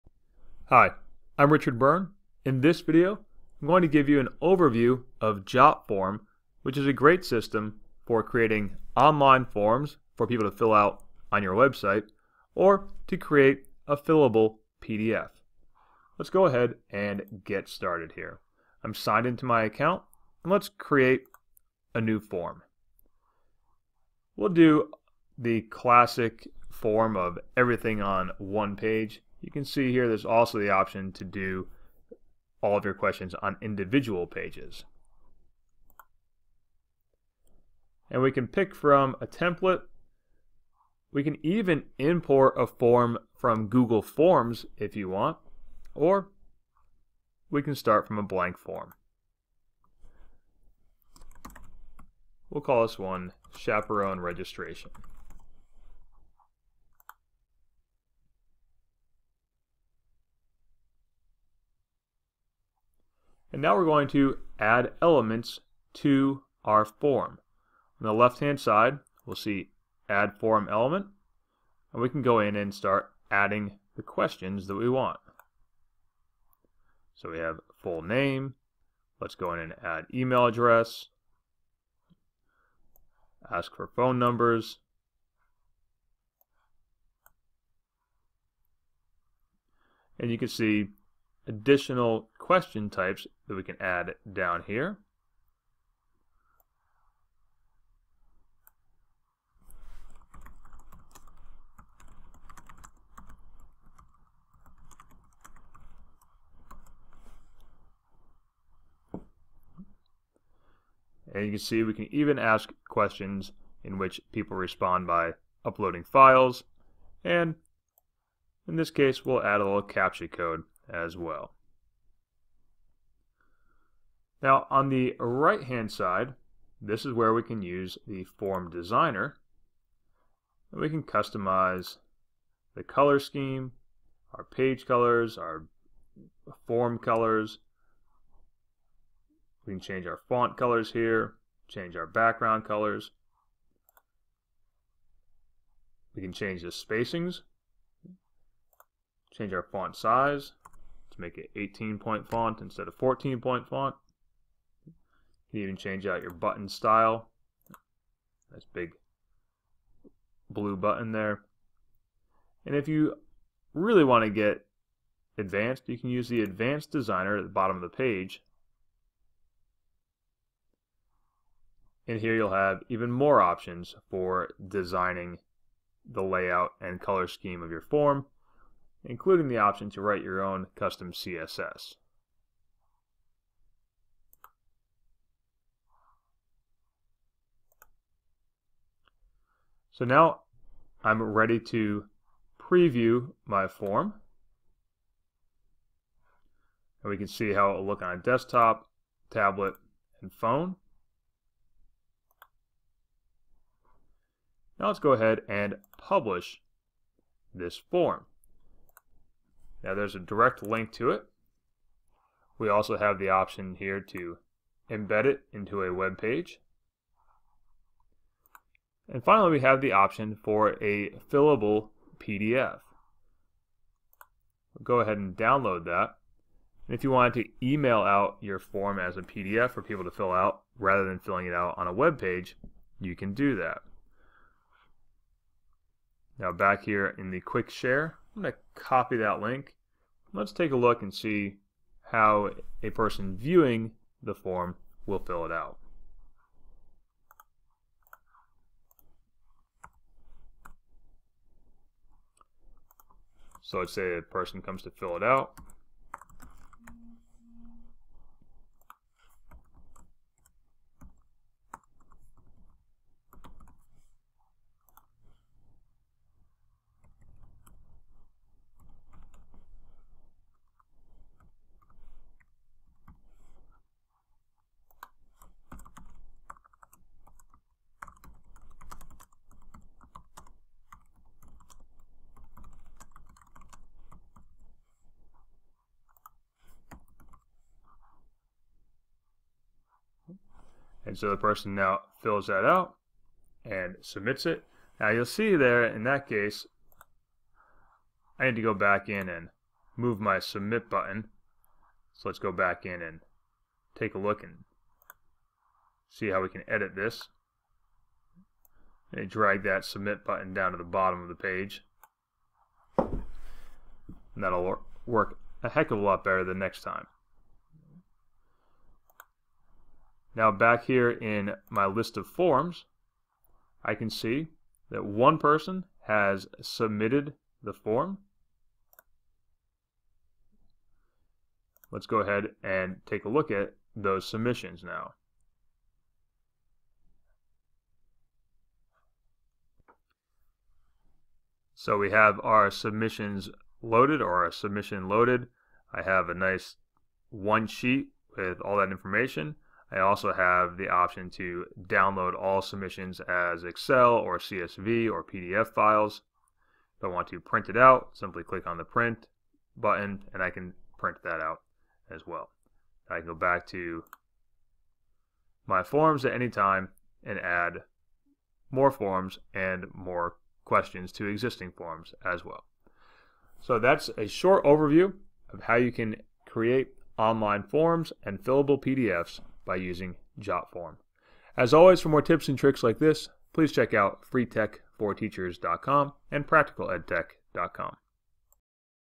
How to Use JotForm to Create Online Forms & Fillable PDFs
JotForm is a good tool for making online forms and fillable PDFs. It offers design features and information collection options that you can't find in Google Forms.